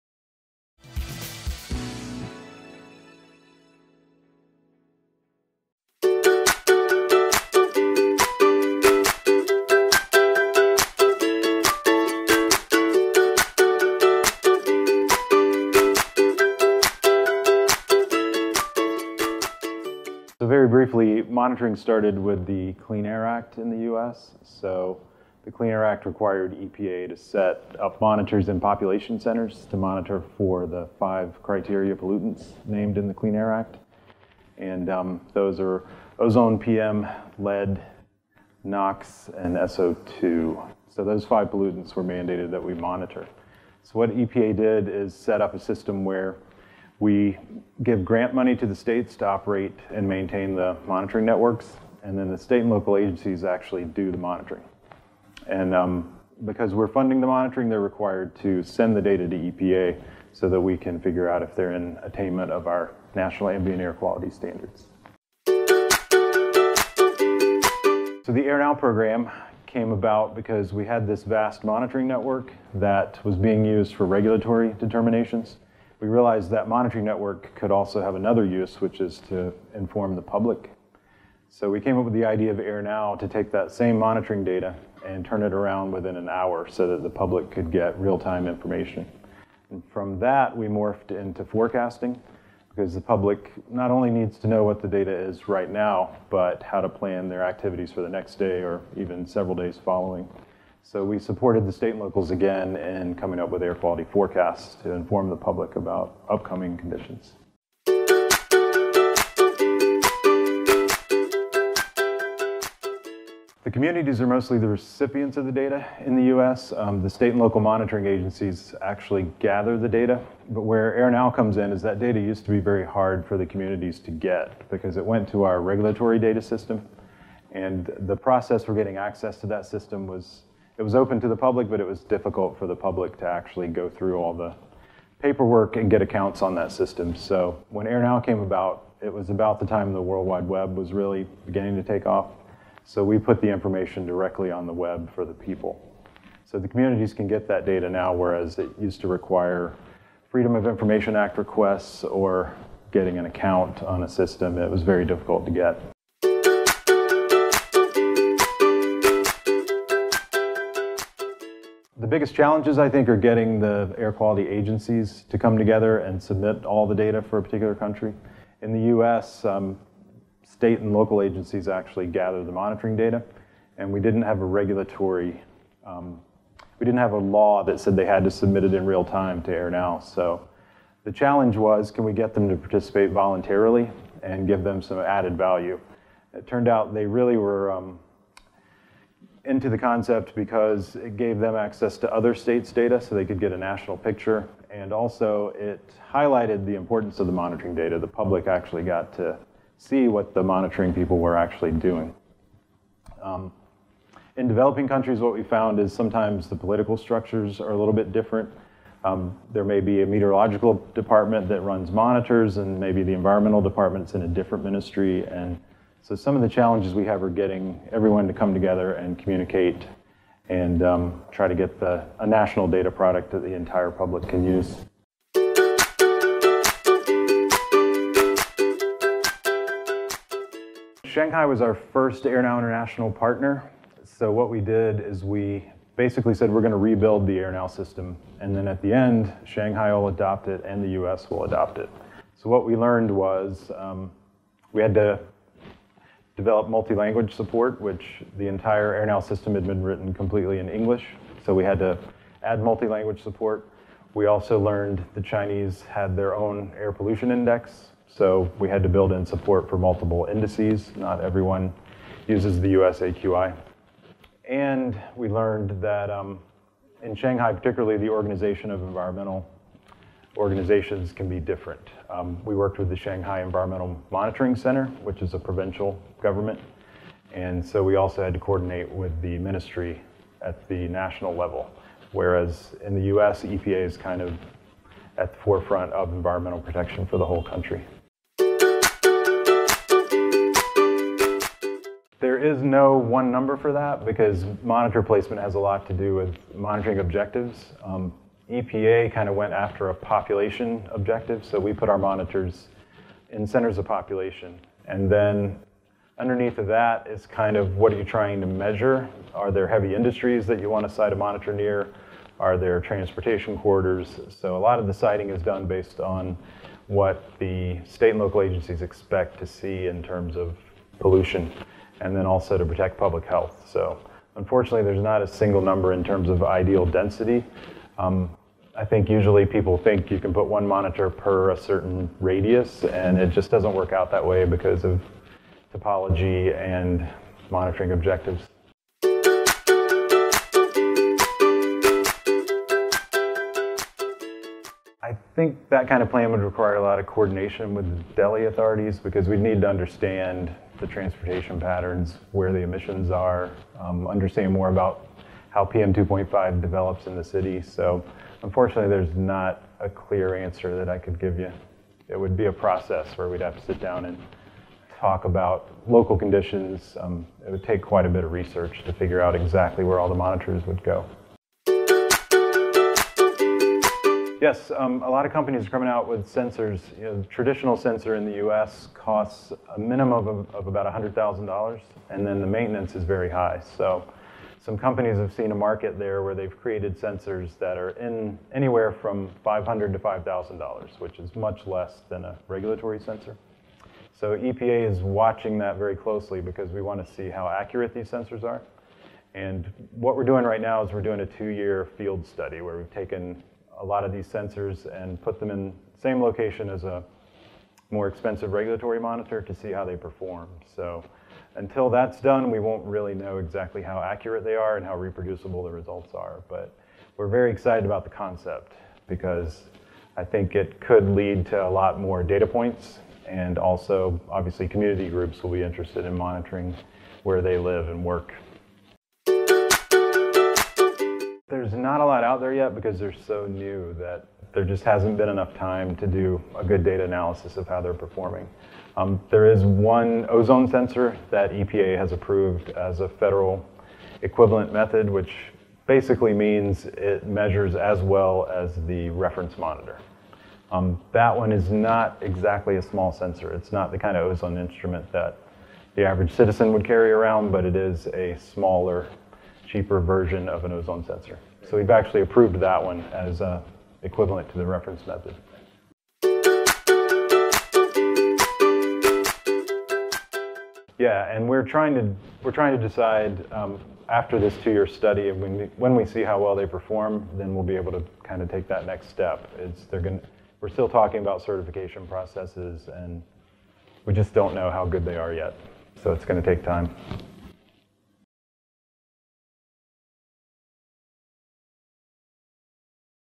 Air Pollution Monitoring Technologies: Interview with Phil Dickerson
Phil Dickerson is director of AIRNow program at US Environmental Protection Agency.